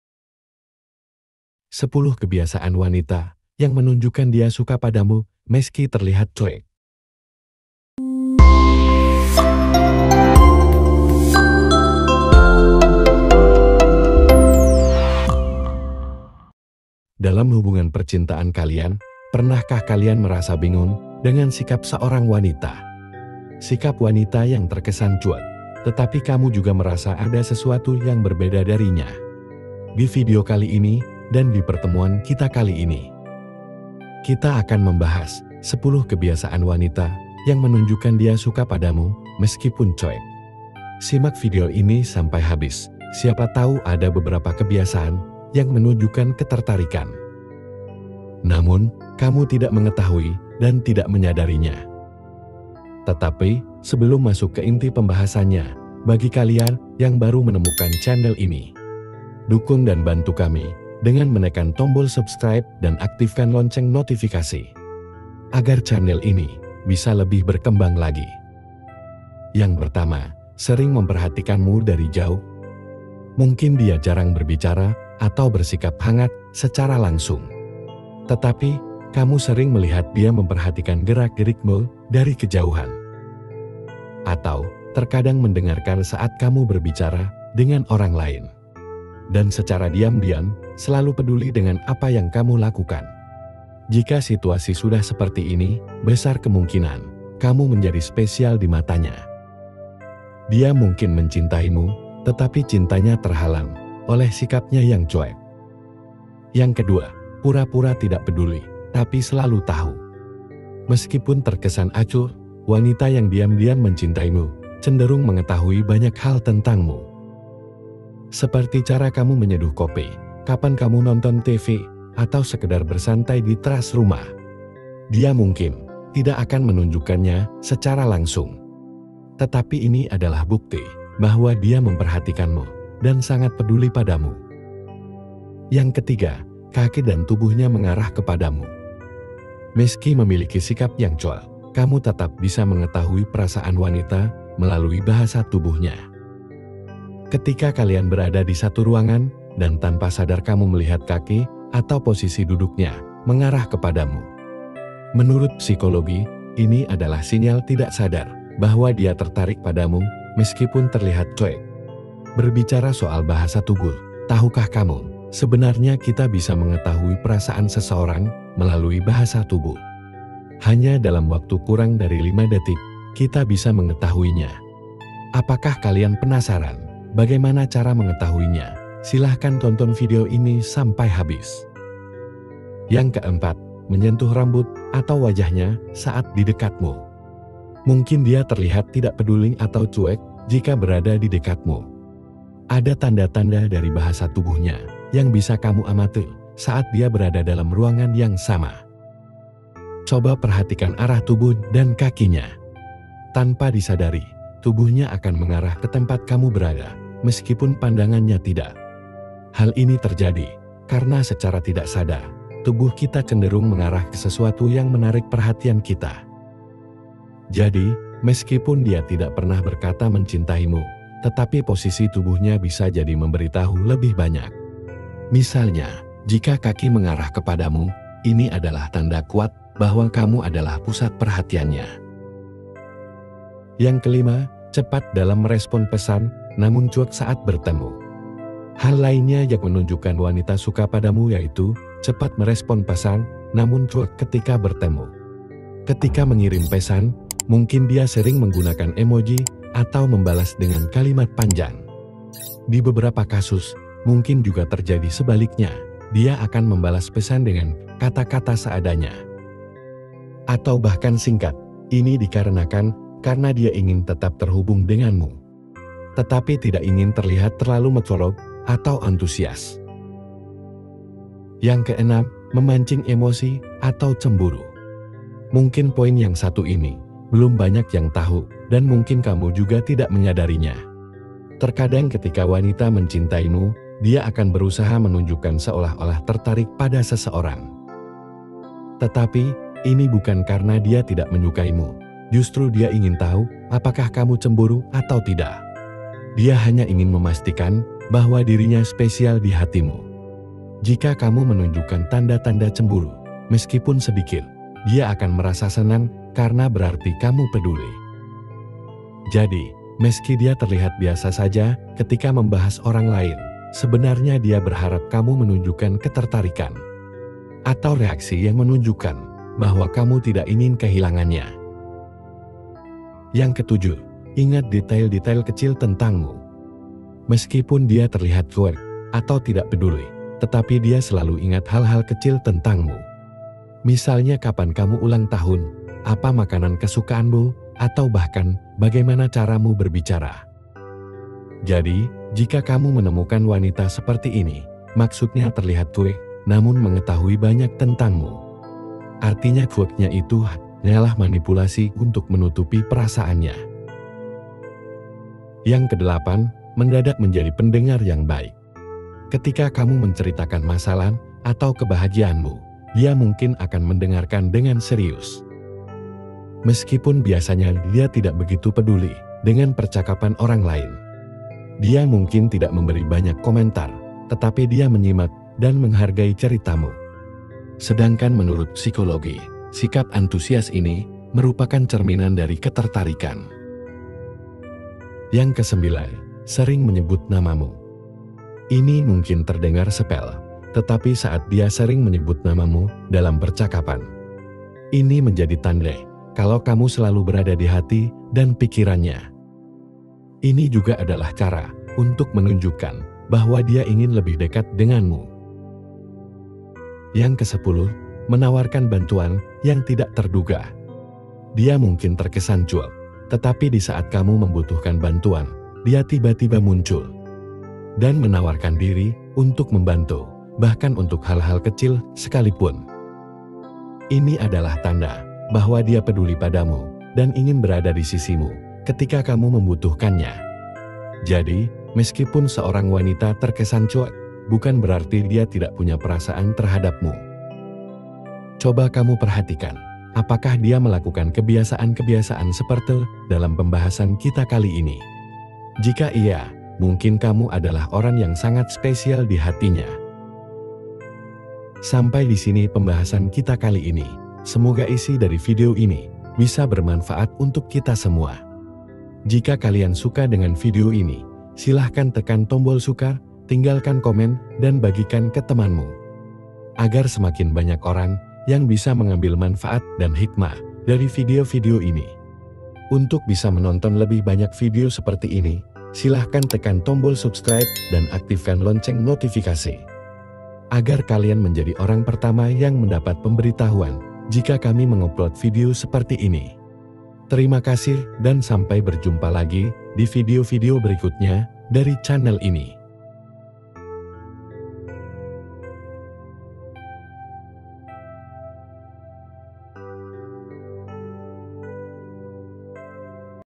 ciri wanita cuek !! ||10 Kebiasaan Wanita Yang Menunjukkan Dia Mencintaimu Meski Terlihat Cuek
10 Kebiasaan Wanita Yang Menunjukkan Dia Mencintaimu Meski Terlihat Cuek
Dalam hubungan percintaan kalian, pernahkan kalian merasa bingung akan sikap seorang wanita yang kamu cintai.
Sikap wanita yang terkesan cuek, tetapi kamu juga merasa ada sesuatu yang berbeda darinya.
Pada pertemuan kita kali ini atau di dalam video ini, kita akan membahas sikap wanita yang dimaksud dala video yang berjudul : 10 Kebiasaan Wanita Yang Menunjukkan Dia Mencintaimu Meski Terlihat Cuek.
Adapun sikap wanita yang terkesan cuek tersebut adalah :
1. Sering memperhatikanmu dari jauh
2. Pura-pura tidak peduli tetapi selalu tahu
3. Kaki dan tubuhnya mengarah kepadamu
4. Menyentuh rambut atau wajahnya saat di dekatmu
5. Cepat dalam merespon pesan, namun cuek saat bertemu
6. Memancing emosi atau cemburu
7. Ingat detail-detail kecil tentangmu
8. Mendadak menjadi pendengar yang baik
9. Sering menyembut namamu
10. Menawarkan bantuan yang tidak terduga
Tonton video ini sampai habis dan jangan di skip, siapa tahu ada sikap dari pasanganmu yang sama dengan pembahasan di video kali ini, yang mungkin kamu tidak menyadarinya.
Semoga pembahasan kita kali ini bisa bermanfaat bagi kita semua.
Terimakasih dan selamat menonton.